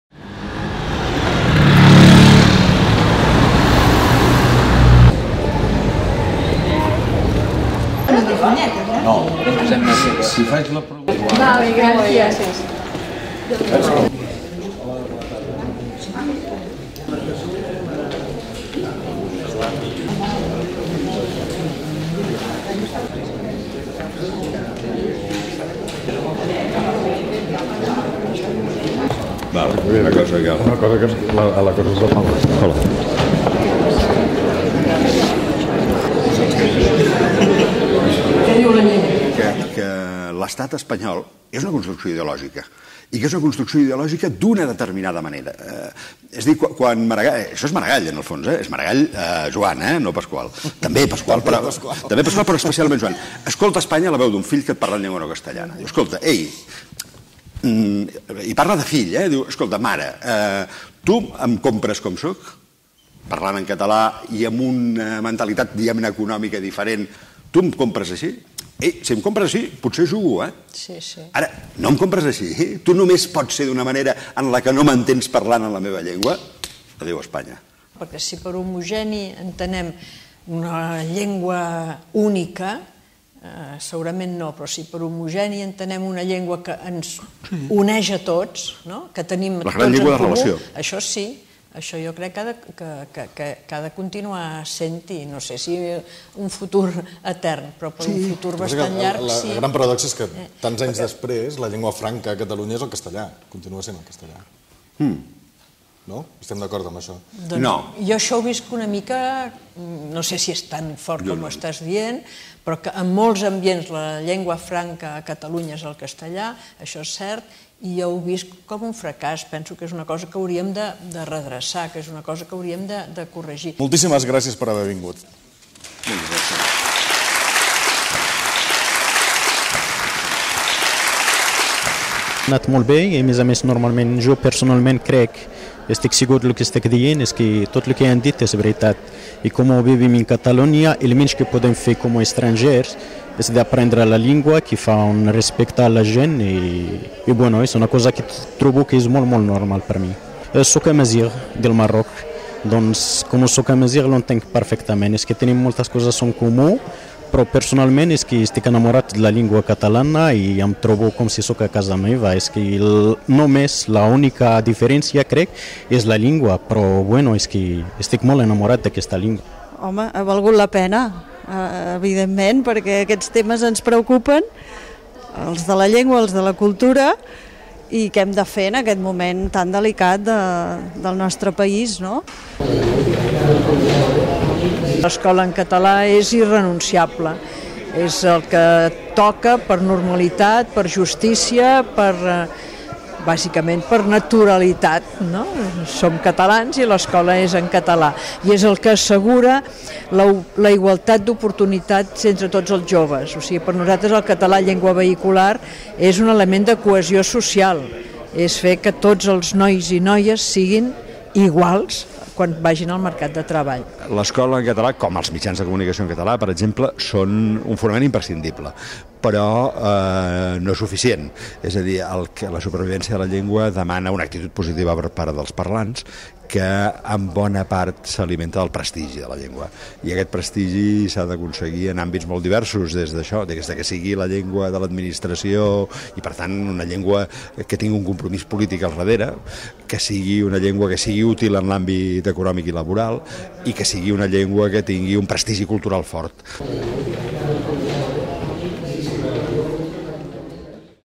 Nació Digital - Idees per a avui (3). Vicenç Villatoro i Muriel Casals
Tercer debat del cicle Idees per a avui organitzats per Nació Digital i La Torre del Palau. Conversa sobre la llengua catalana entre Vicenç Villatoro i Muriel Casals